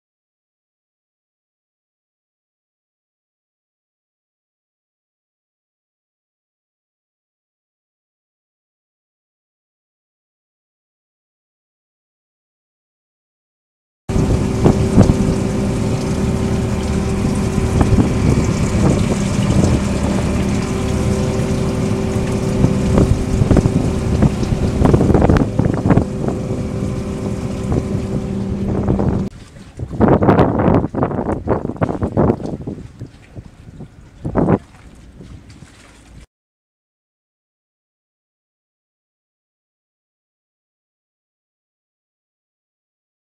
꽁치가 물풀 진저리에 산란한 꽁치알을보기가 요즘은 진짜 귀하다
꽁치가 산란을할때 육지가 가까운 진저리라는 물풀에 알을 낳는다
이 진저리는 파도에 뿌리가 뽑혀 떠다니는데 꽁치가 지나가다 알을 낳은 물풀이 바다 물조류에 밀려다니다 문어통발 기촉부위에 걸렸다
예전에는 많많은알들이 떠 다녀서 주워다 삶아먹기도했다
그런데 수년전부터 꽁치가 귀하니 꽁치알을 구경하기란 참 어렵다
10년전만하여도 꽁치가 많이 잡혀와 항구가 그득 했는데